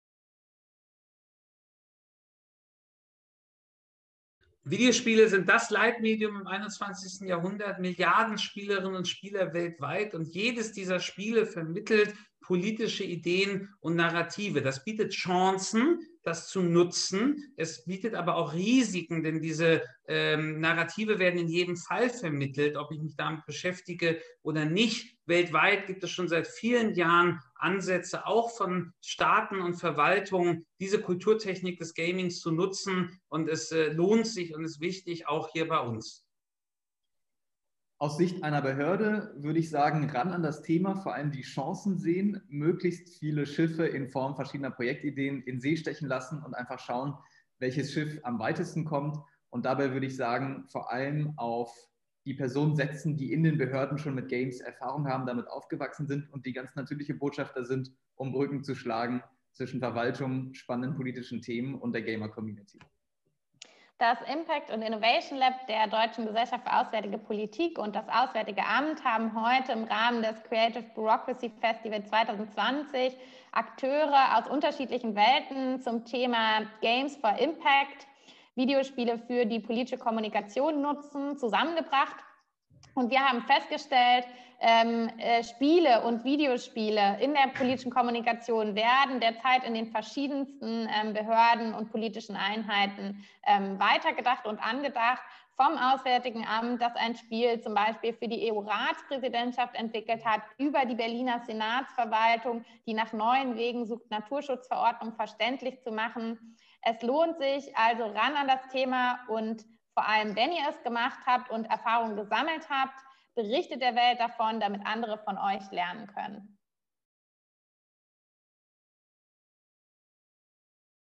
"Games for Impact" - Videospiele für die politische Kommunikation nutzen
Einblick in die Creative Bureaucracy Festival Academy Session vom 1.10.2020
mit Manouchehr Shamsrizi & Claire Luzia Leifert vom DGAP Impact and Innovation Lab und Kaspar Meyer vom Auswärtigen Amt

Videospiele sind das meistgenutztes Kulturmedium der Gegenwart. Sie sind Treiber von technologischen Innovationen (AI, Blended Reality, Connectivity) und vermitteln politische Narrative und Weltbilder.

Vor dem Hintergrund des zunehmend hart geführten Wettbewerbs der Narrative in der internationalen Politik führt dieser Workshop in das Potential von Videospielen als Instrument der politischen Kommunikation ein.

Die Teilnehmenden erhalten Einblicke in den Entwicklungsprozess des für die deutsche EU-Ratspräsidentschaft entwickelten Videospiels „Pathways – Europe at your Fingertips“ und entwickeln eigene Ideen für den Einsatz von Videospielen in ihrem Arbeitsfeld.